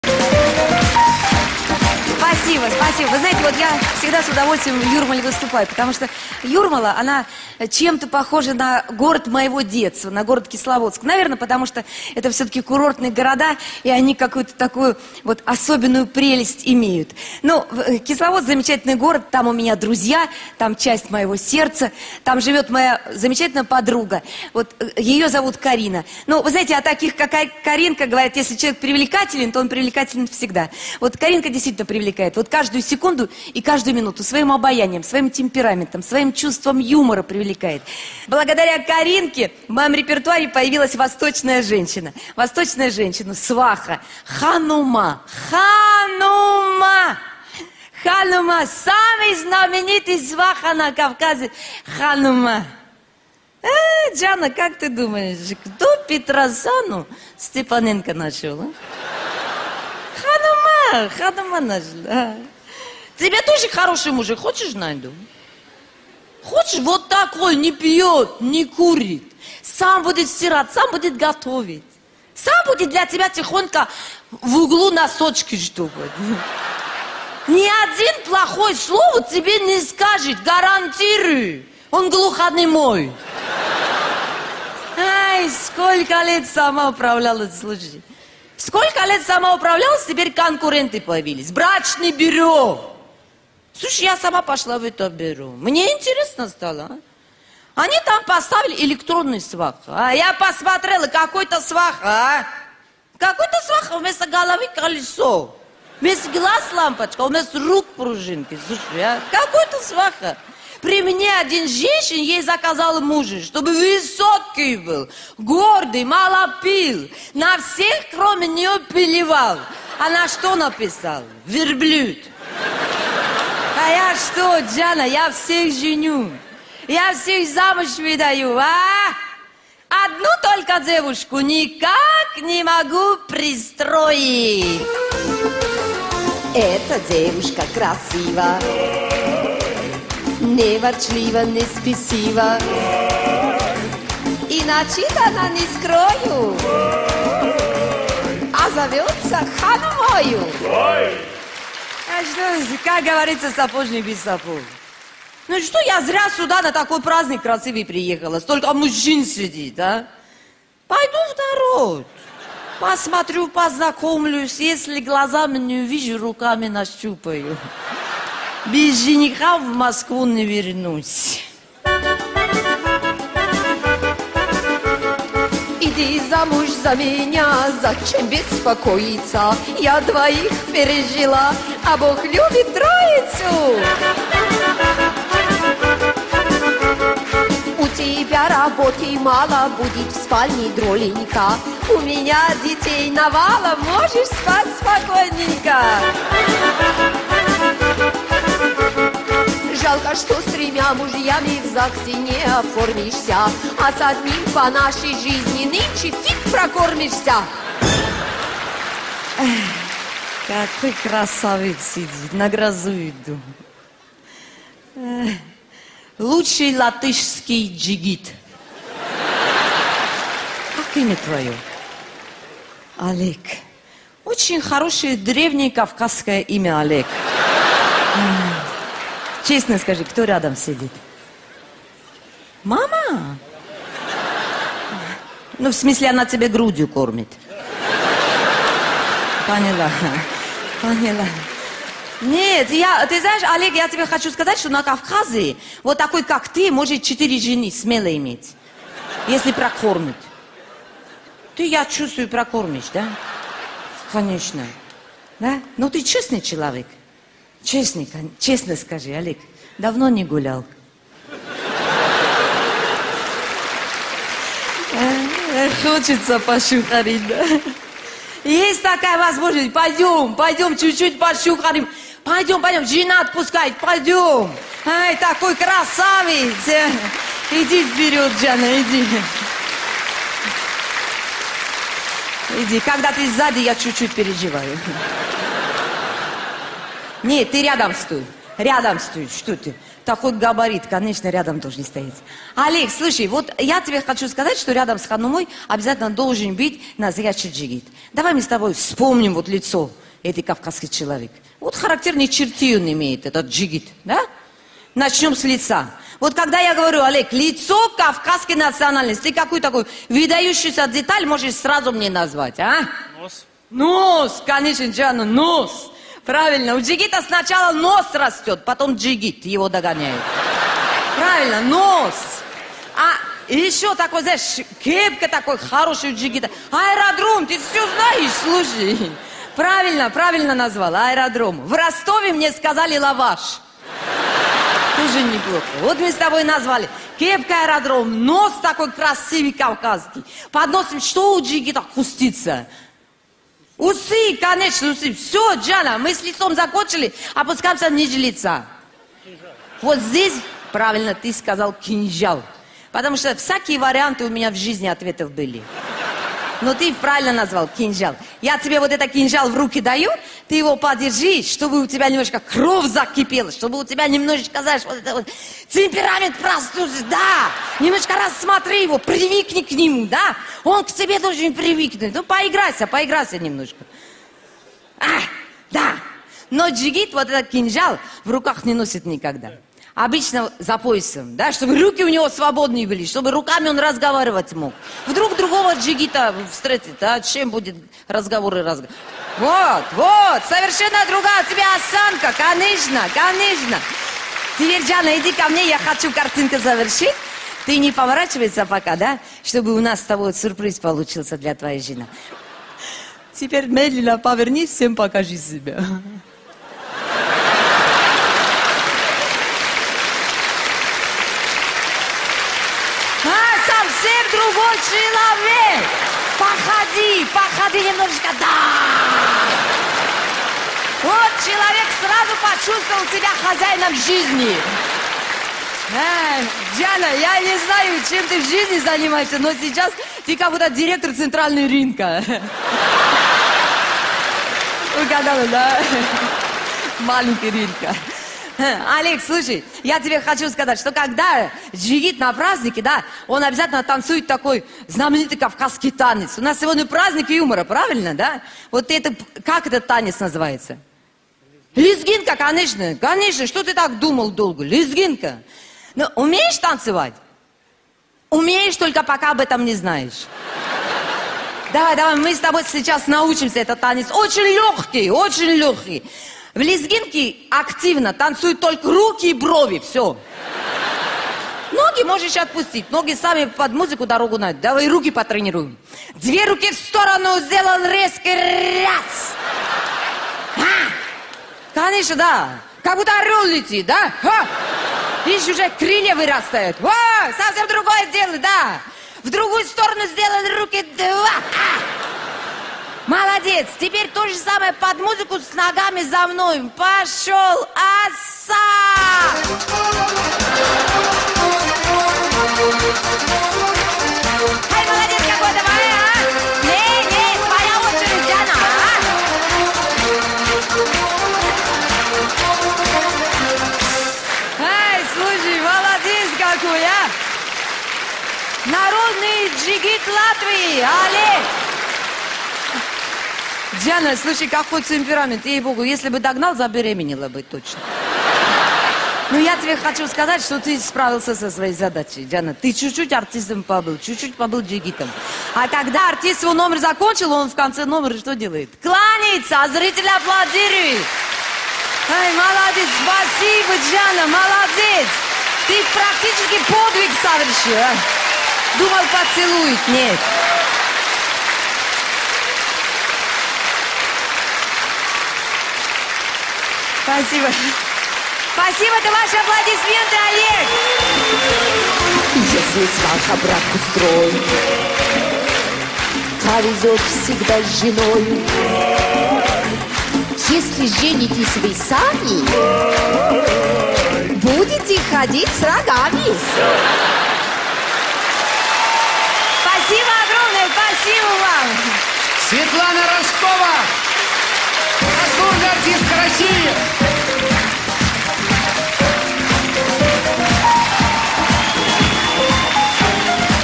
Светлана Рожкова - Сваха Ханума
Фестиваль Юмора в Юрмале 2010

Светлана Рожкова в соц.сетях, присоединяйся и будь в курсе самых свежих новостей!
Гастроли, съемки и будни заслуженной Свекрухи страны!